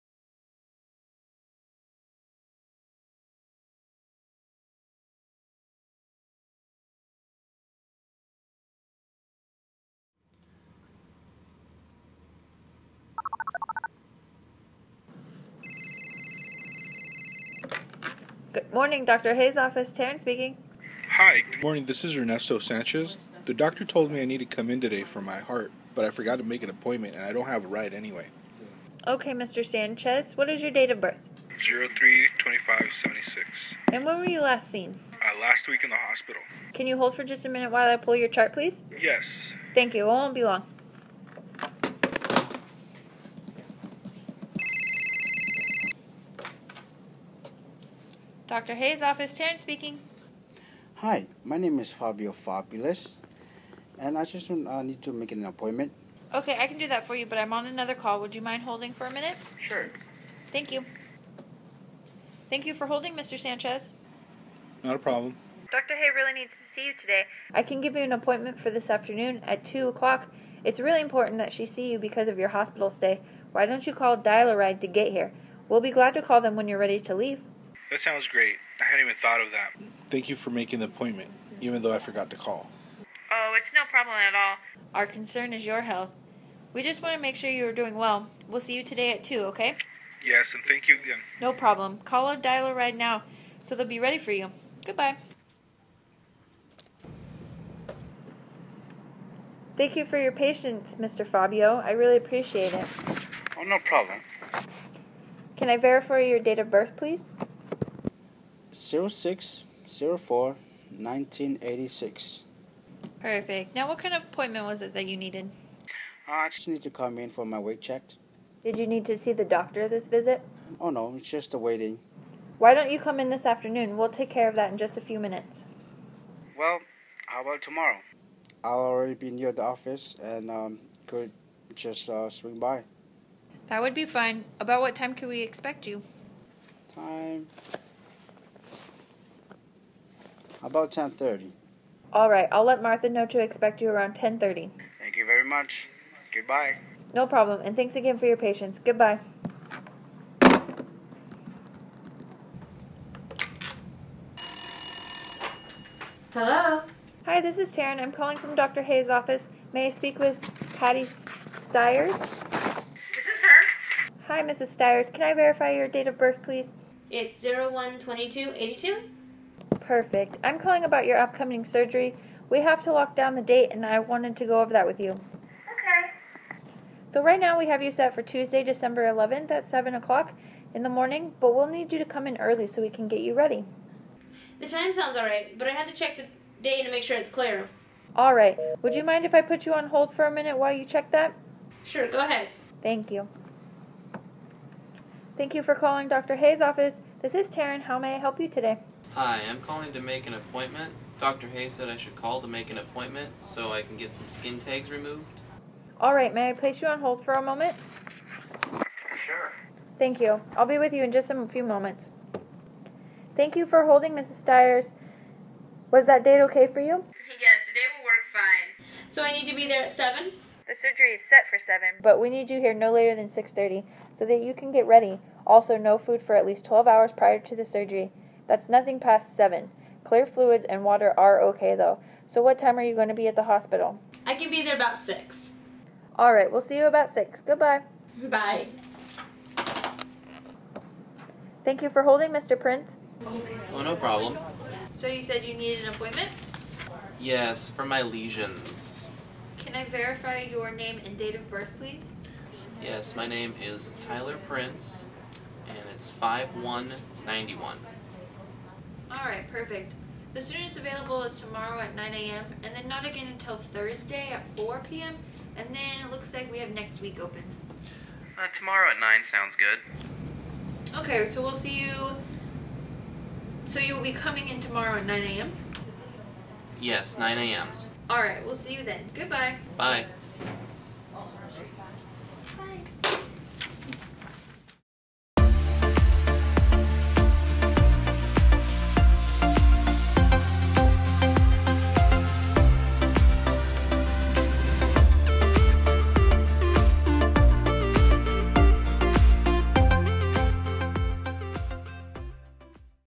How A Medical Assistant Should Answer the Phone
This is how you should answer a multiline telephone in a medical office.